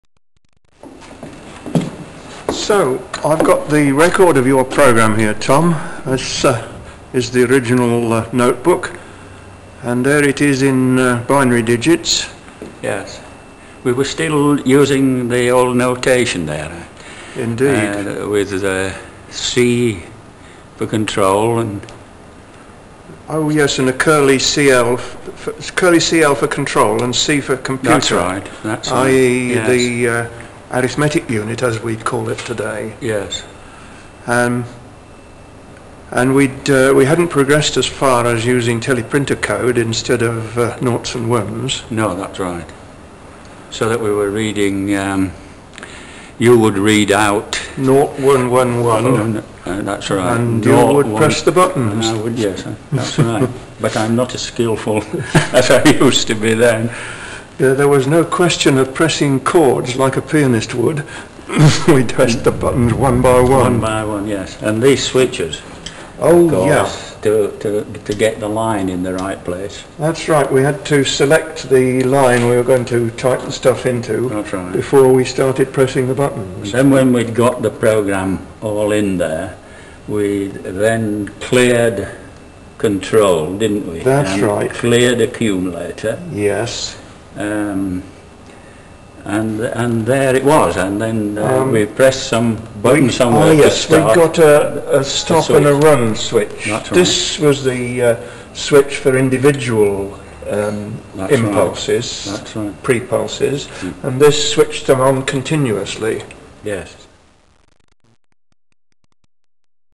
Manchester Baby Computer
Talk by two of the inventors of the Manchester computer on how the system would be used.